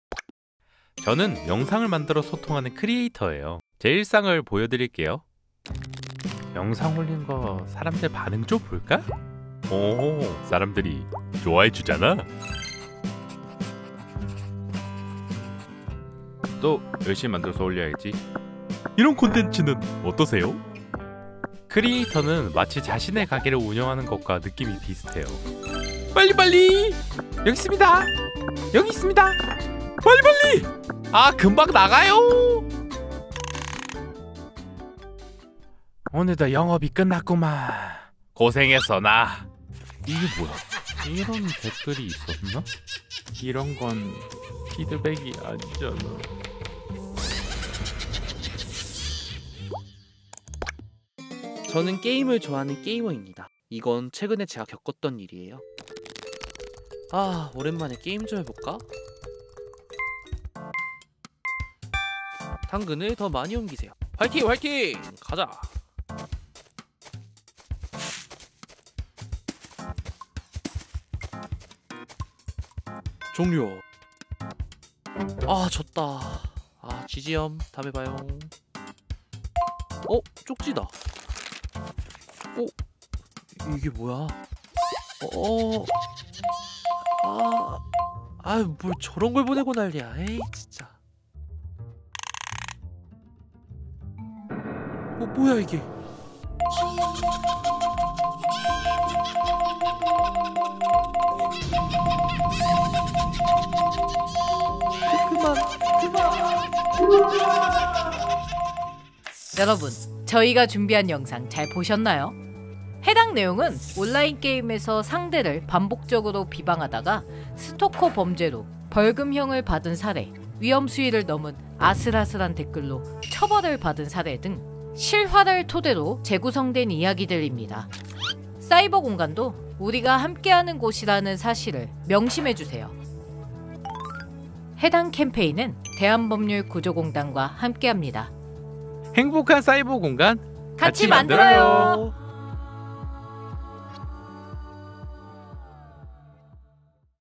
사이버범죄, 당신의 온라인 공간은 정말 안전한가요?
안전하고 행복한 사이버공간을 만들기 위해 우리 모두 함께 노력합시다!